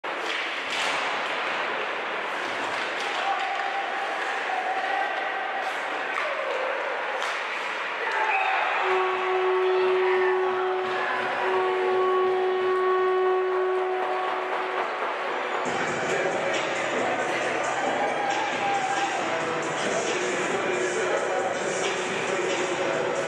Teague Vader scores his first junior goal with the Listowel Cyclones
Vader buried his first GOJHL goal early in the 2nd period on Sunday November 5th for the Cyclones.

Listowel beat Caledon 10-0 at the Steve Kerr Memorial Complex. We'll have a full game recap later on 519!